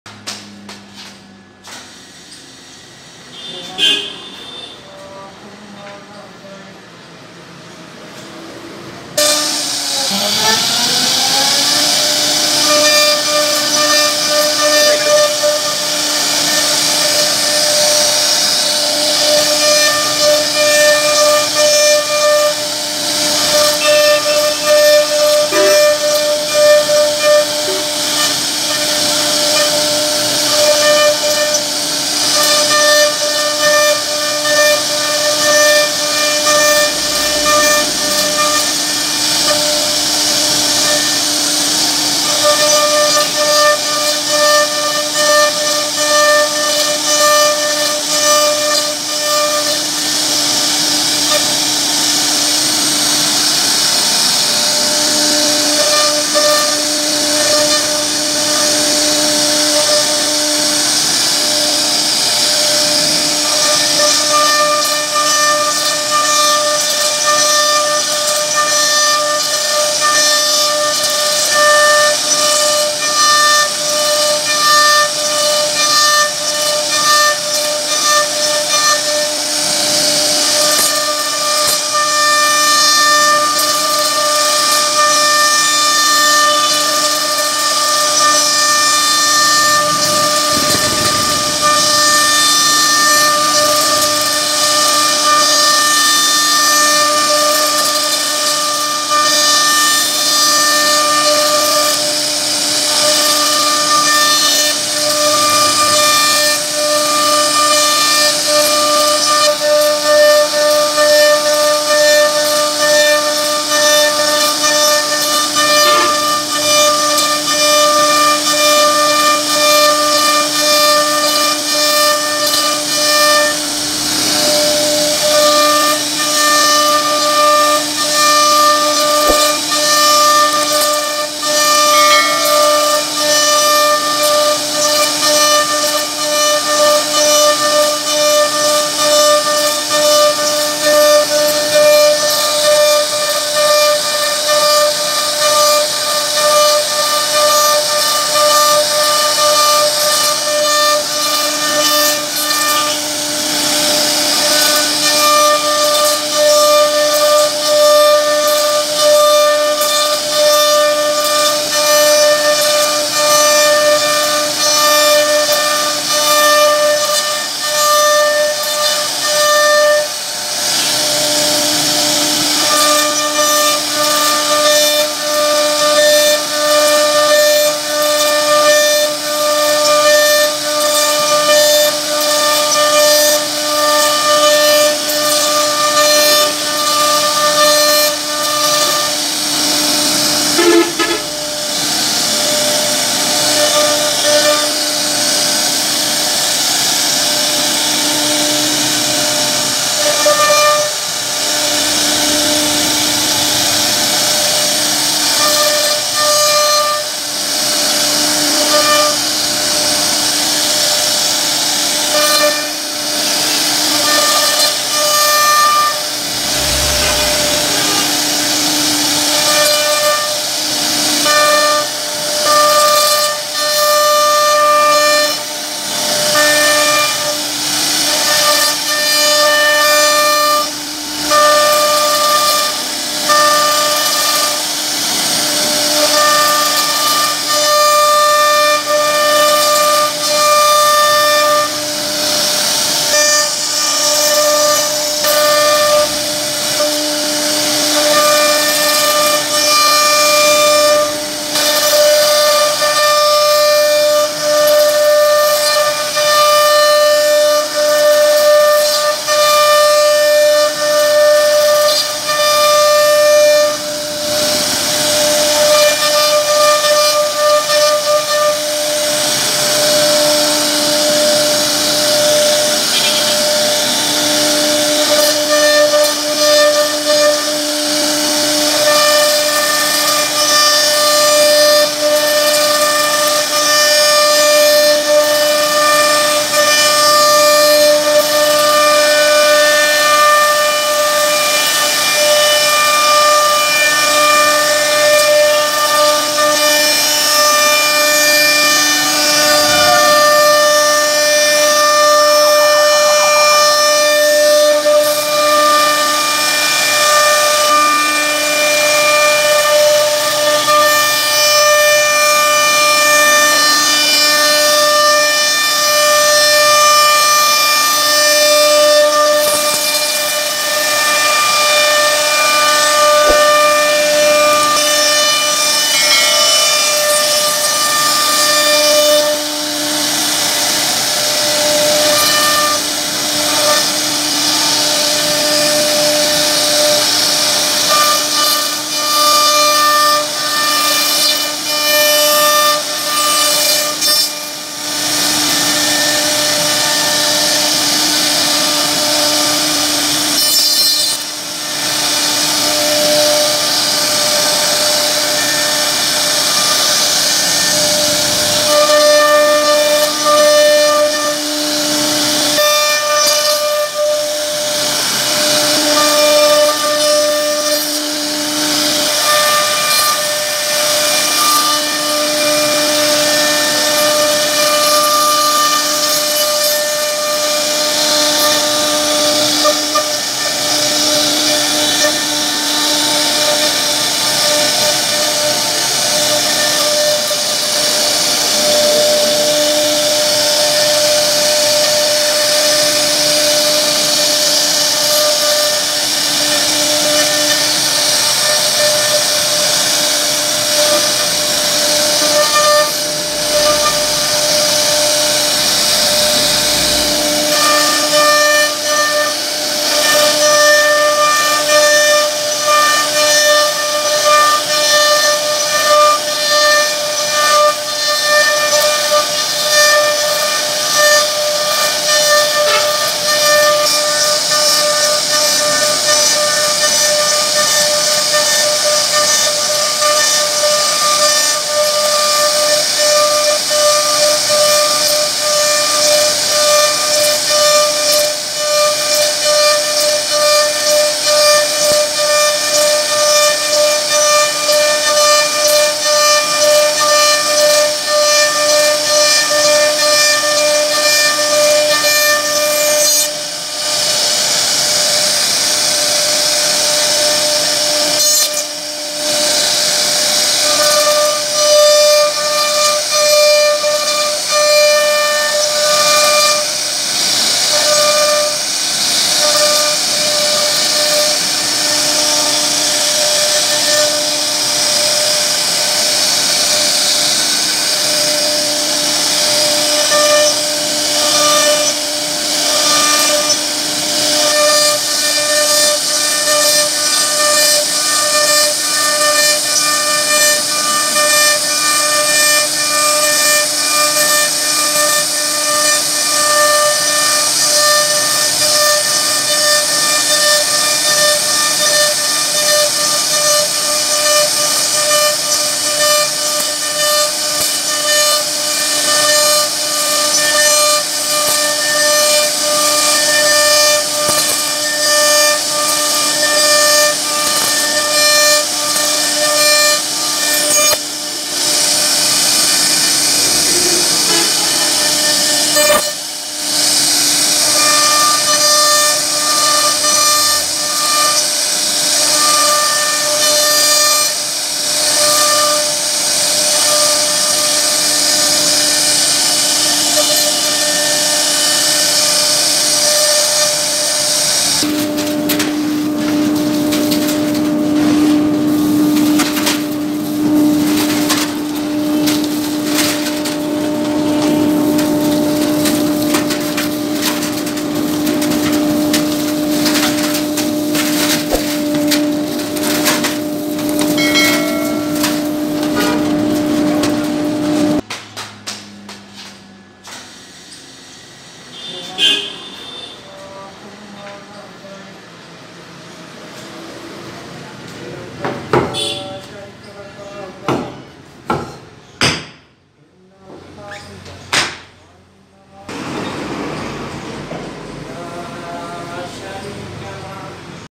Easy Creative Wooden Bed Pocket Designing by CNC Router Machine || Royal Bed Design Beautiful way
Perfect so easy creative wooden bed pocket designing CNC router machine. Most wonderful royal bed design in the world
at a glance.It's high quality royal bed design very beautiful way.Most amazing wooden modern bed design for bedroom.The beautiful wooden design is carved on a plain wood board to make a smart bed.
This design for home images can help you to select perfect design for your house. CNC wood router can be used for panel production, furniture, door, cabinet, etc.